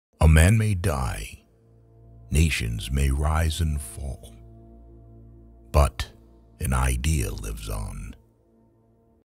John E. Kennedy Quotes Ever || 38 | Motivational Quotes
ohn E. Kennedy was a pioneering advertising copywriter. In 1904, Kennedy began working with Albert Lasker at the Chicago office of advertising firm Lord & Thomas where he became the highest paid copywriter in all of advertising.